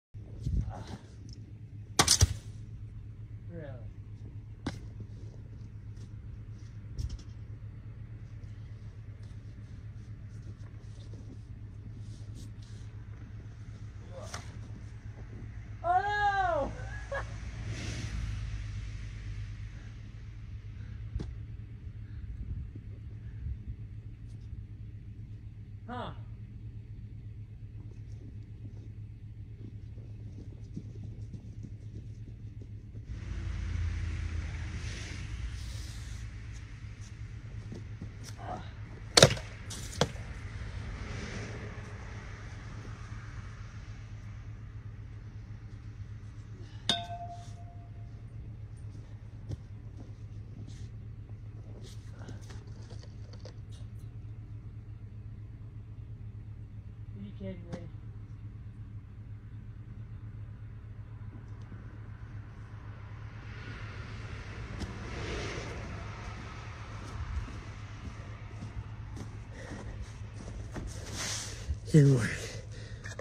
A ladder or bigger shovel?
Ever have one of those days where you end up throwing your shovel on the roof? Yeah, me neither, but let's imagine! 😂 There you are, standing in your yard, looking up at your shovel chilling on the roof like it's sunbathing in the Bahamas.

Now what? Call it a new weather vane? Start a trend? 🤷‍♂️ Here's a life lesson, folks: Sometimes we toss our 'shovels' in the most unexpected places. Instead of scratching our heads, let's laugh, grab a ladder (or a bigger shovel), and deal with it.

Life's too short not to find humor in the bizarre.